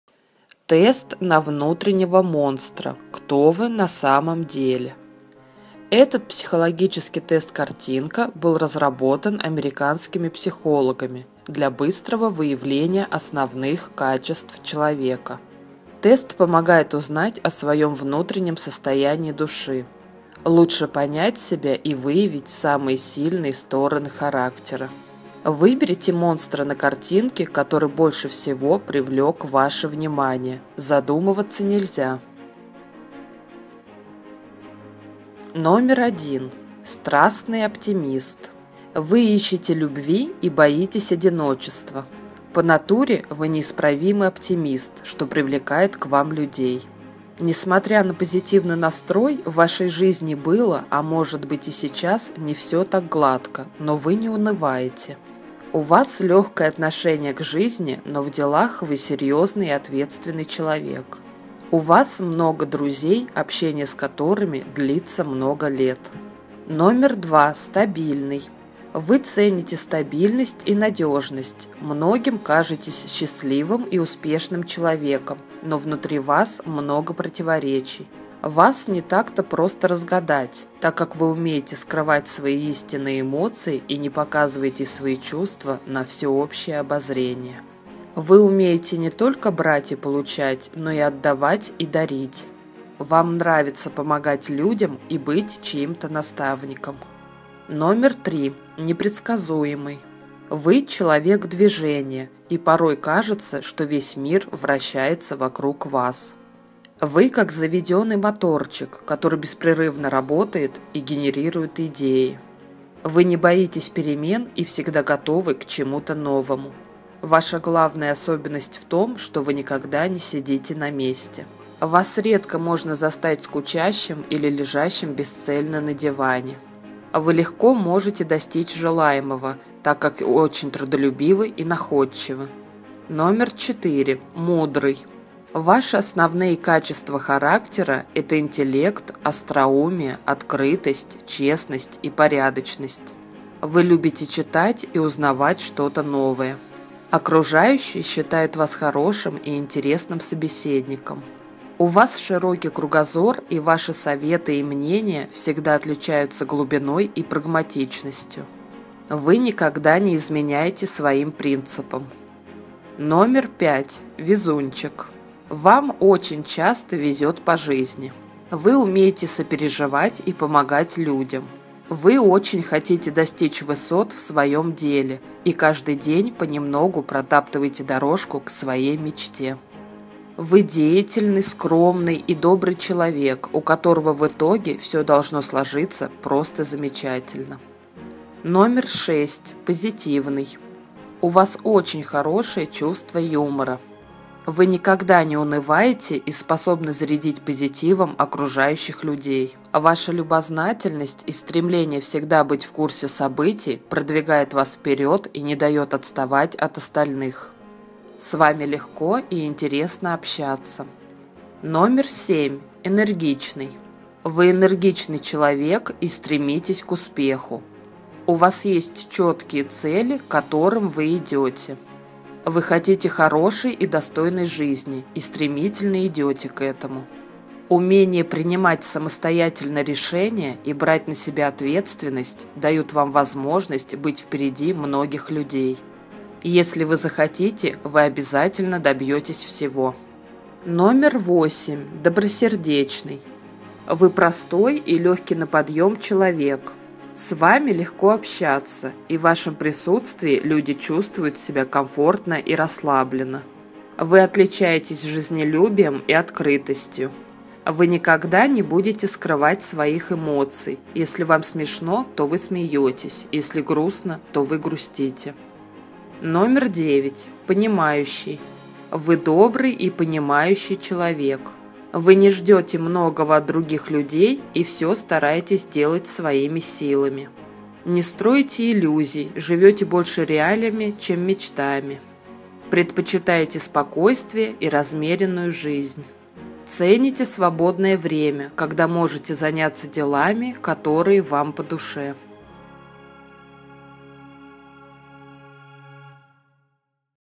Тест на внутреннего монстра: кто вы на самом деле?
Этот психологический тест-картинка был разработан для быстрого выявления основных качеств человека...Выберите монстра на картинке, который больше всего привлек ваше внимание! Задумываться нельзя!